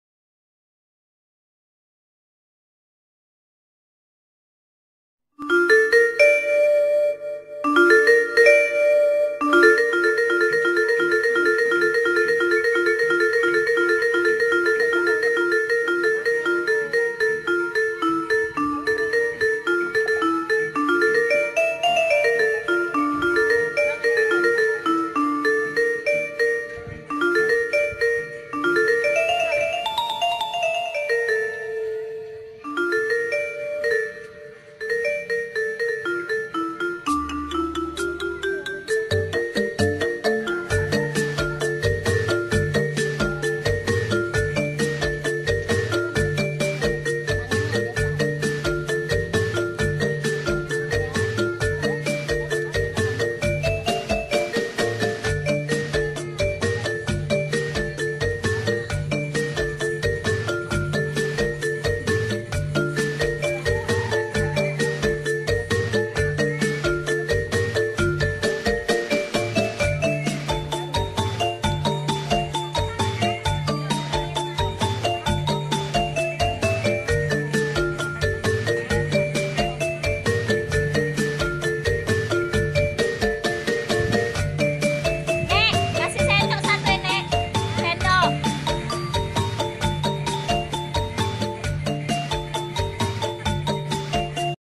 GOTONG ROYONG의 인도네시아 문화)KEC,BESITANG kab LANGKAT SUMUT
GOTONG ROYONG EMAK" Masi는 마을의 공동체에서 재해와 사건의 관점에서 훌륭한 시민을 돕는 데 순수합니다.kec besitang KAB LANGKAT SUMUT